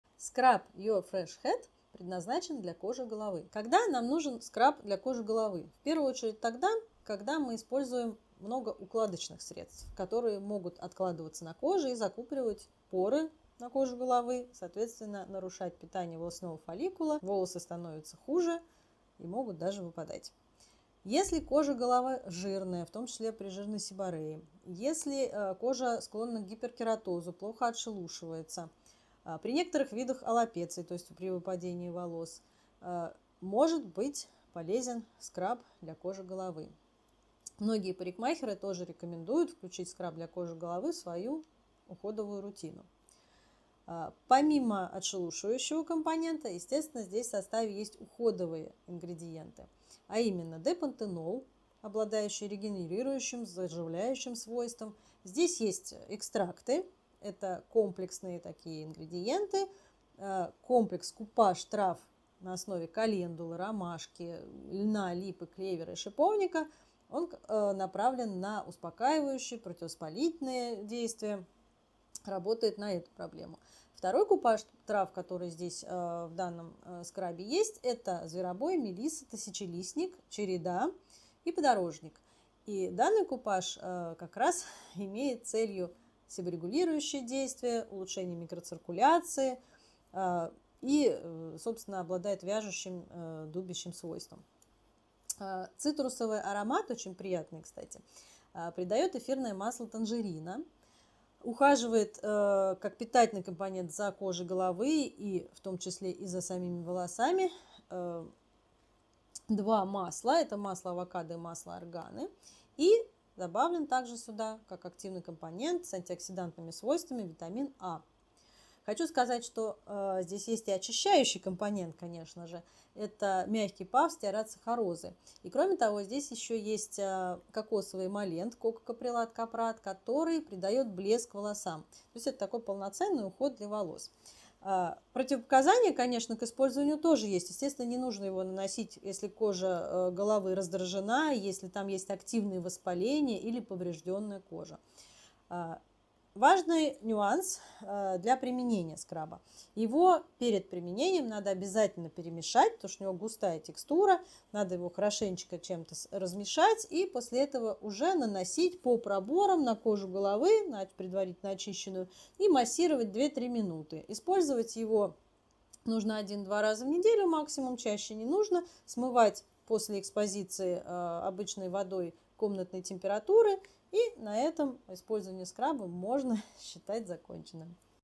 Скраб для волос Your fresh head
Натуральный скраб для кожи головы на основе соли послужит отличным дополнением в уходе за волосами. Регулярное скрабирование кожи головы принесёт сразу несколько видимых результатов: уменьшение сальности кожи головы и ускорение роста волос за счёт улучшения микроциркуляции. Мельчайшие соляные кристаллы способствуют глубокому, при этом деликатному очищению кожи головы от излишков кожного сала, стайлинга и других загрязнений. Д-пантенол отвечает за смягчение и регенерацию эпидермиса. Масла авокадо и арганы интенсивно питают кожу головы без утяжеления и потери прикорневого объема. Специально разработанный микс экстрактов лекарственных трав, таких как календула, ромашка, липа, клевер, шиповник, подорожник, череда и мелисса успокаивают и оказывают себорегулирующее и противовоспалительное действие. Скраб не царапает и не повреждает кожу головы, легко и полностью смывается, не оставляя скрабирующих частиц по всей длине, а его цитрусовый аромат вполне заменит процедуру ароматерапии и улучшит настроение.